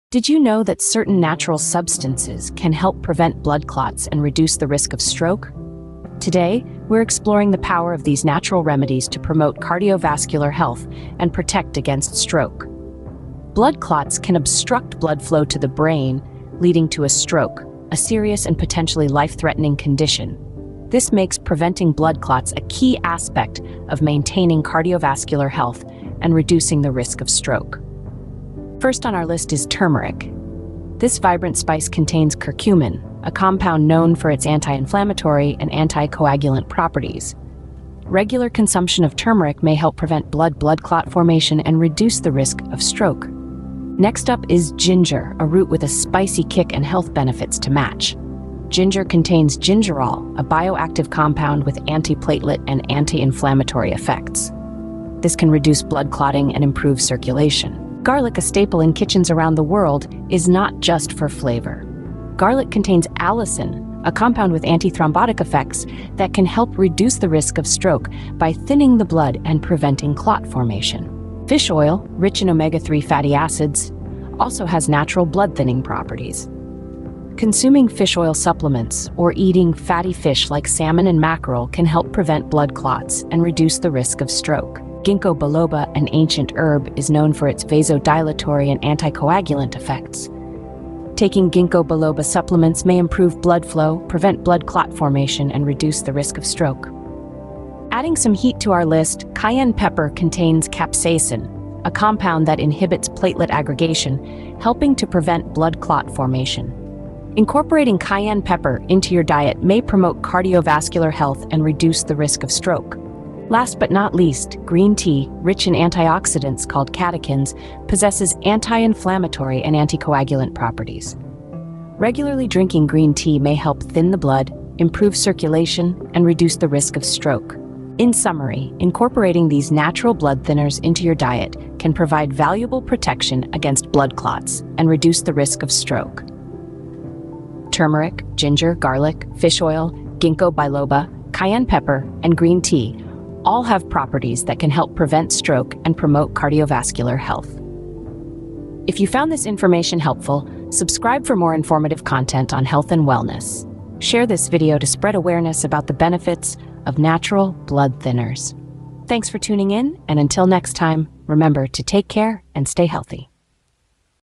Natural Blood Thinner: Preventing Stroke
In this video, we will explore the benefits of natural blood thinners in preventing strokes. Discover how incorporating these natural remedies into your lifestyle can help maintain healthy blood flow and reduce the risk of clot formation. Learn about the science behind these natural blood thinners and their effectiveness in promoting cardiovascular health. Let's delve into the various natural options available and how they can contribute to stroke prevention. Let's get dive into timestamps to examine each natural blood thinner's efficacy and how to integrate them into your daily routine for optimal health.

Timestamps:

0:00 - Introduction to Blood Clots and Stroke
0:29 - Turmeric- Nature's Anti-Inflammatory
0:44 - Ginger-Spicy root with health benefits
0:58 -Garlic-More than just a flavor Enhancer
1:12 - Fish Oil-Omega 3 fatty acids for blood thinning
1:25 - Ginkgo Biloba-Ancient herb for blood flow
1:38 - Cayenne Pepper-Heat for Cardiovascular health
1:53 - Green Tea-Antioxidants for blood thinning
2:08 - Conclusion and call to action

As we conclude, we've explored the significance of natural blood thinners in preventing strokes and promoting cardiovascular health. Together, let's prioritize our cardiovascular health and take proactive steps towards a healthier future.